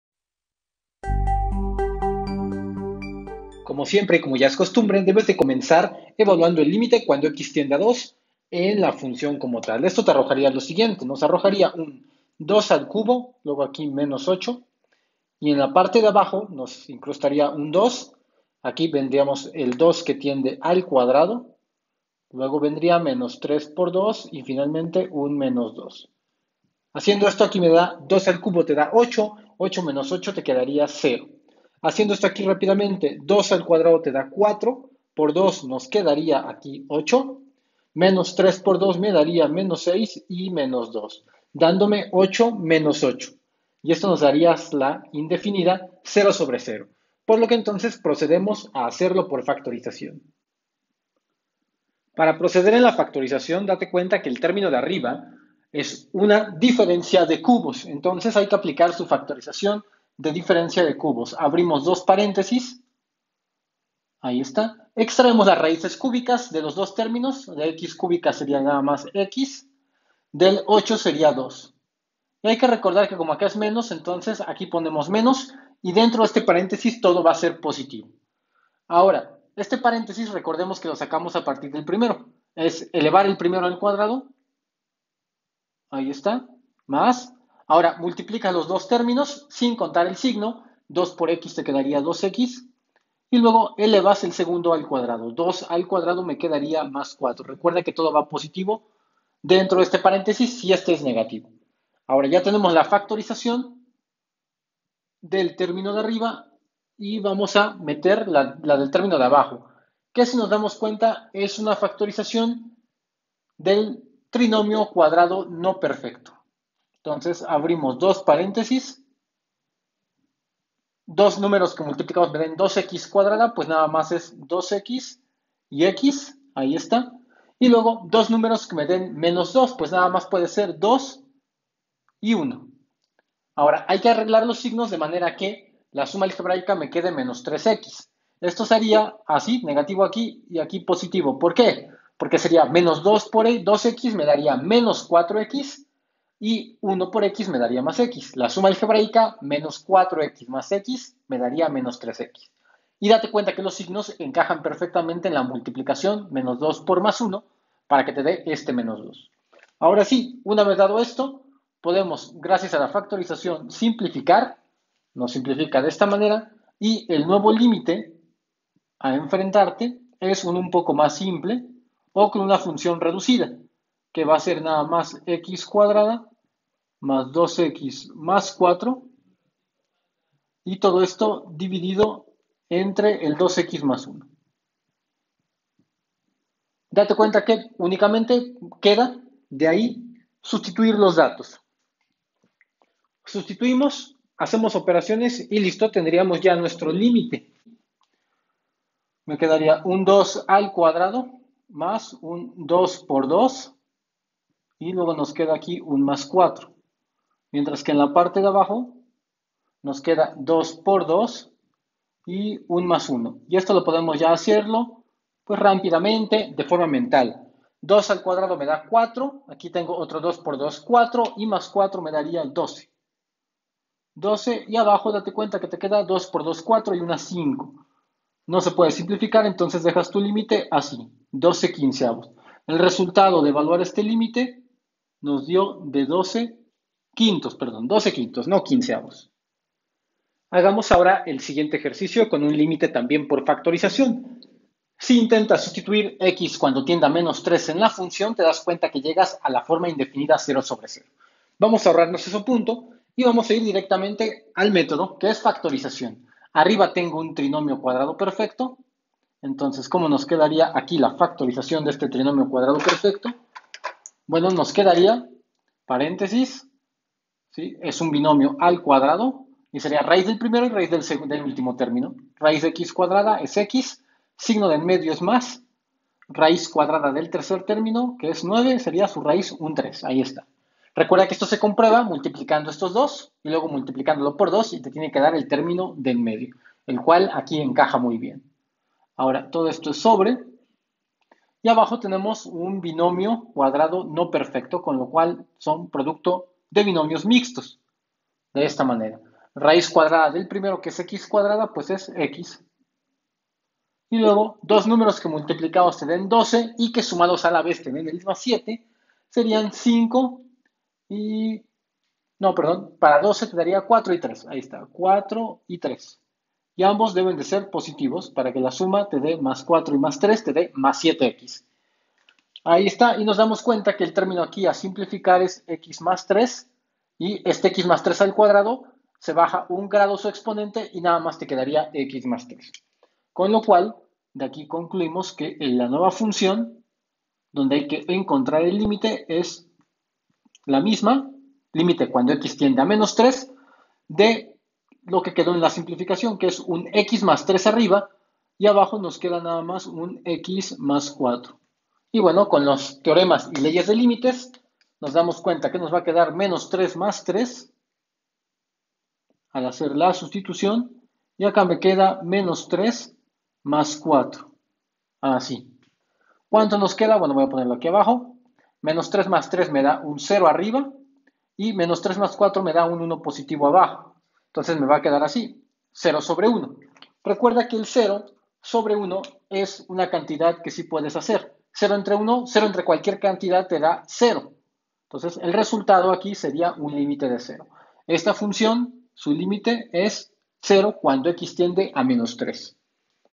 ✅ 𝐋𝐈𝐌𝐈𝐓𝐄𝐒 𝐏𝐎𝐑 𝐅𝐀𝐂𝐓𝐎𝐑𝐈𝐙𝐀𝐂𝐈Ó𝐍 [ℙ𝕒𝕤𝕠 𝕒 ℙ𝕒𝕤𝕠💪🏻💯😎] Cálculo Diferencial
Límites por Factorización Parte 2

🔽Descarga PDF con link a todos los vídeos del canal, para que estudies tema por tema.

¿Quieres más beneficios de nuestro contenido?
Hazte MIEMBRO del canal da clic al botón "unirte"

Visita nuestra tienda online de productos con temática sobre matemáticas, física y química.